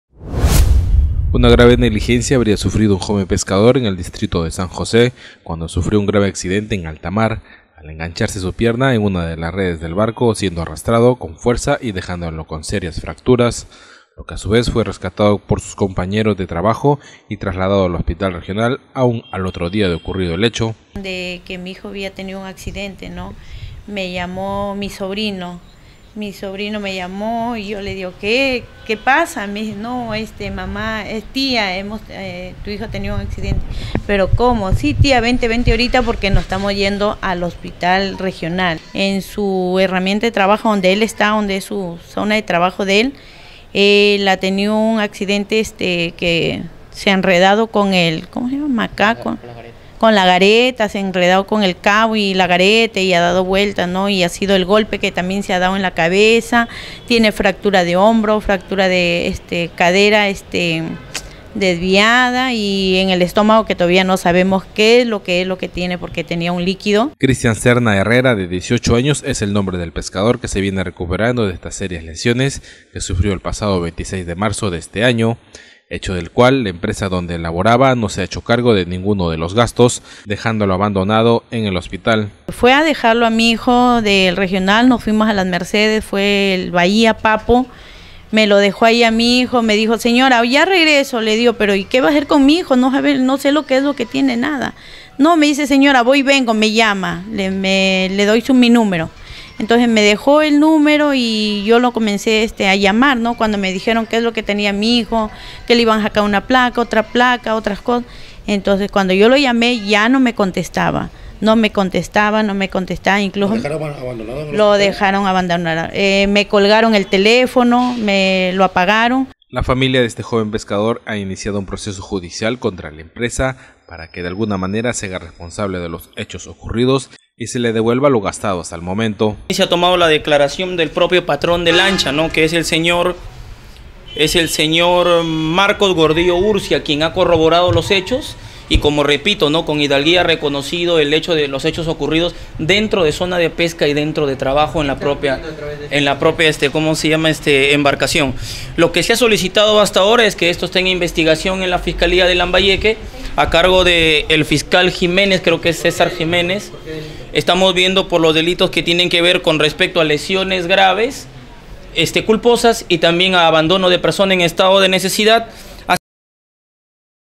Chiclayo: Joven pescador denuncia abandono tras sufrir accidente
Una grave negligencia habría sufrido un joven pescador en el distrito de San José, cuando sufrió un grave accidente en altamar al engancharse su pierna en una de las redes del barco, siendo arrastrado con fuerza dejándolo con serias fracturas. Él denuncia ahora que la empresa se haga responsable de los gastos de su recuperación.